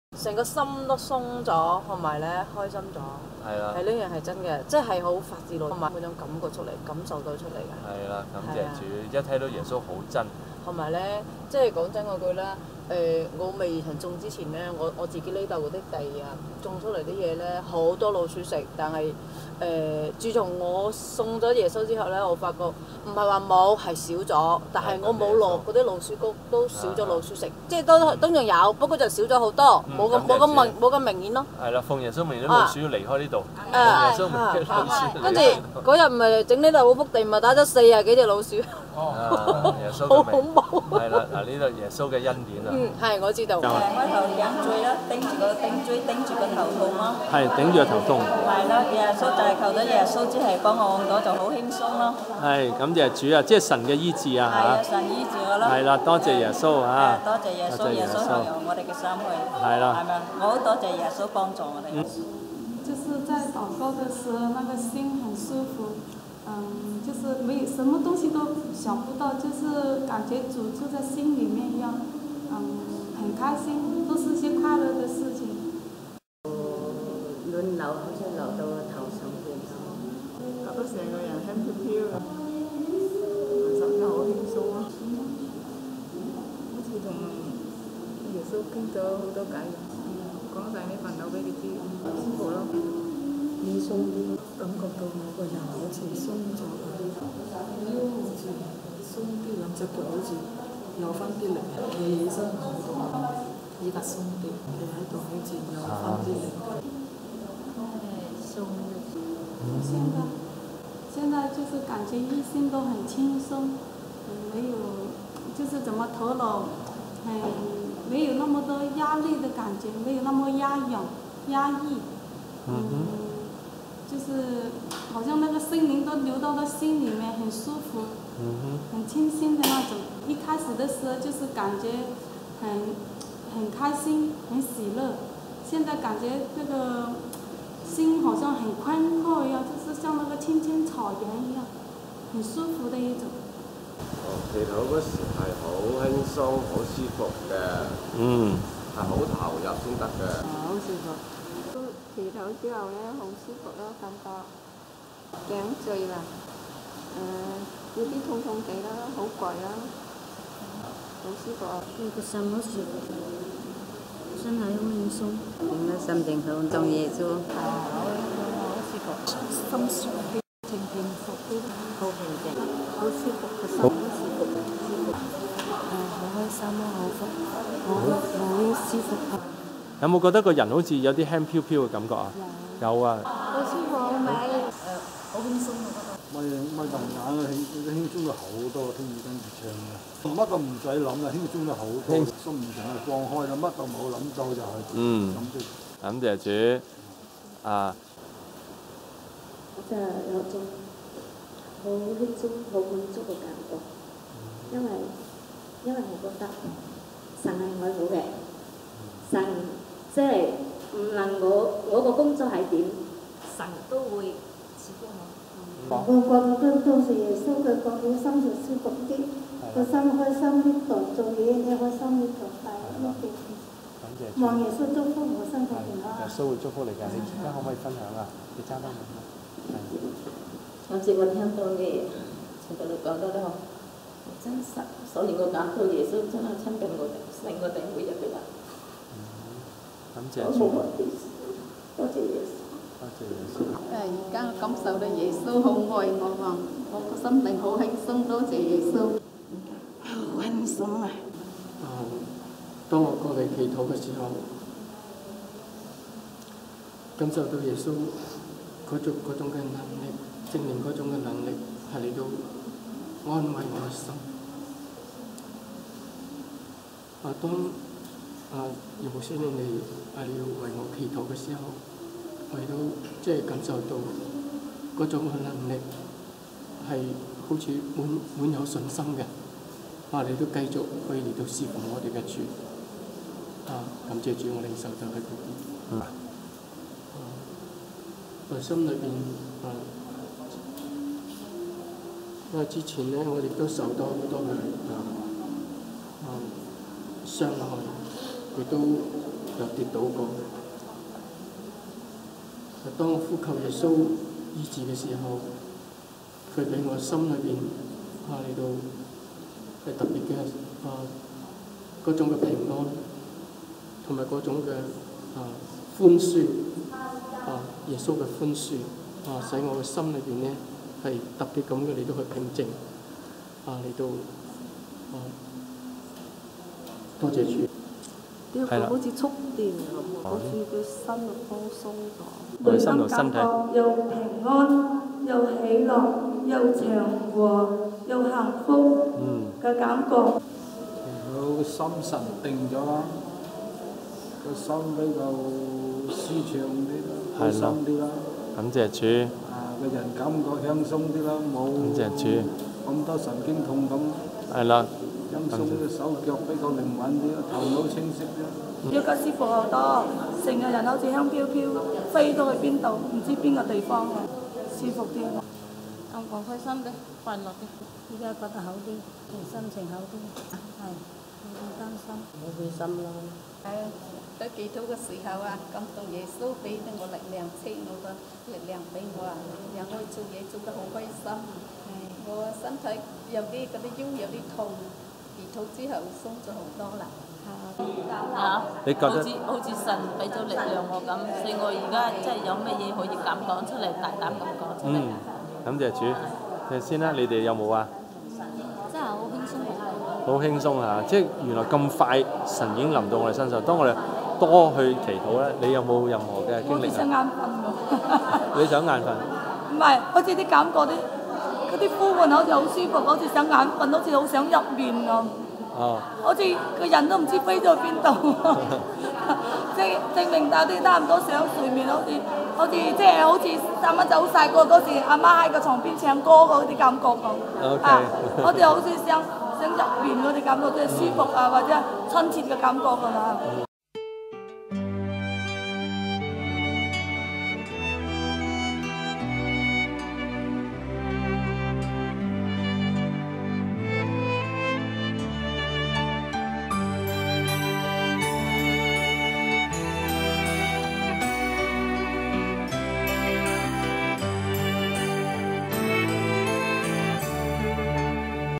不同人分享按手禱告後經歷聖靈-葉牧師
不同人分享按手禱告後經歷聖靈